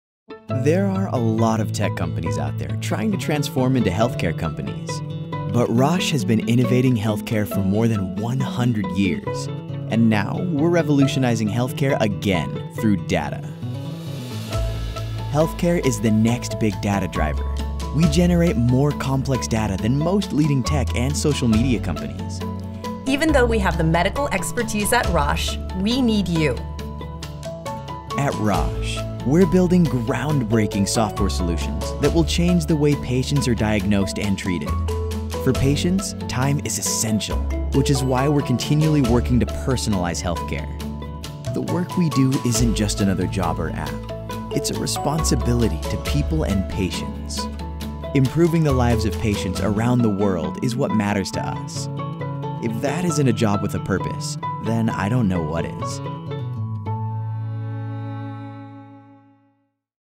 Work with a purpose at Roche
At Roche, we are building groundbreaking software solutions that are changing the way patients are diagnosed and treated. For patients, time is essential which is why we are continuously striving to improve their lives by building personalised healthcare solutions that enable better diagnosis and decisions for their treatments.

Now if that isn’t a job with a purpose, then I don’t know what is – or well, you can of course just develop another app.